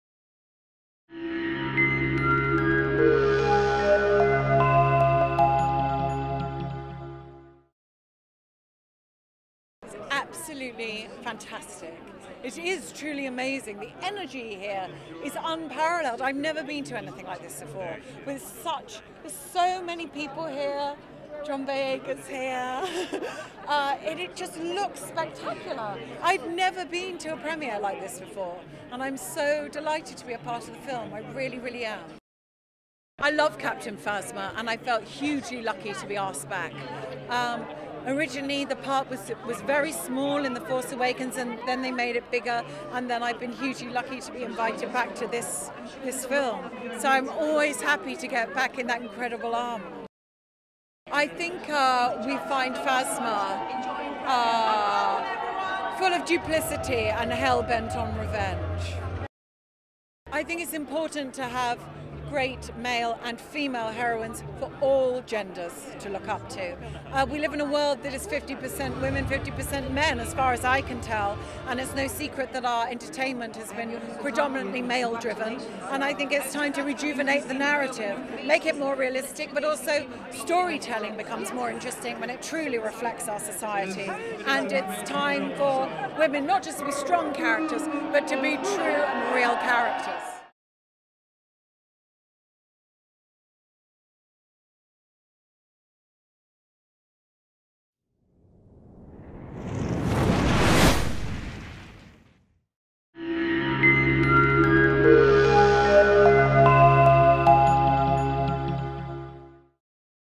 Gwendoline Christie says she has 'never been to a premiere like this before' (The Last Jedi)
STAR WARS: THE LAST JEDI European Premiere was held at the Royal Albert Hall, hosted in aid of the Royal Foundation of the Duke and Duchess of Cambridge and Prince Harry.
Andy Serkis, Anthony Daniels, Benicio Del Toro, Daisy Ridley, Domhnall Gleeson, Gwendoline Christie, John Boyega, Joonas Suotamo, Kelly Marie Tran, Laura Dern, Mark Hamill, Oscar Issac & more were in attendance...